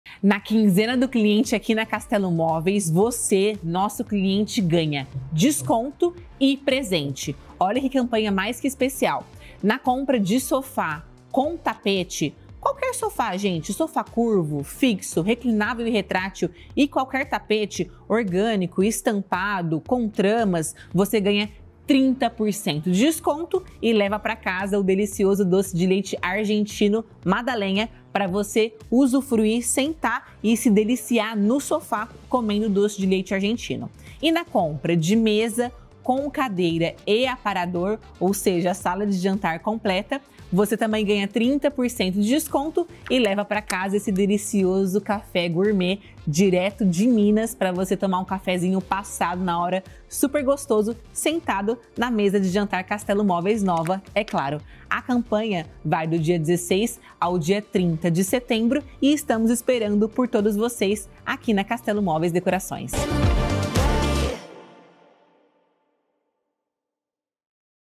🛋️ Promoção Imperdível: Estofados mais Tapete com 30% de Desconto! 🍬
Não perca a chance de renovar sua casa! Neste vídeo, apresentamos os incríveis estofados mais Tapete, com 30% de desconto! E tem mais: na compra de um estofado, você ganha um delicioso doce de leite Magdalena para adoçar ainda mais sua experiência.

✨ Venha conferir as opções e descubra como transformar seu espaço com estilo e conforto.